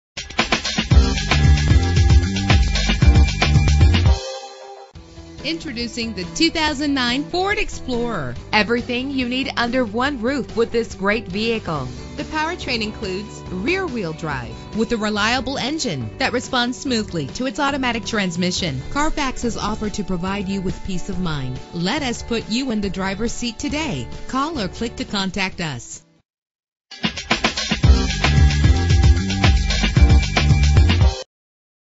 2009 Ford Explorer - Wickliffe OH
Year: 2009
Make: Ford
Model: Explorer
Trim: XLT
Engine: 4.0 L, 6 cylinder
Transmission:
Color: Blue
Mileage: 77437
Address:
28825 Euclid Avenue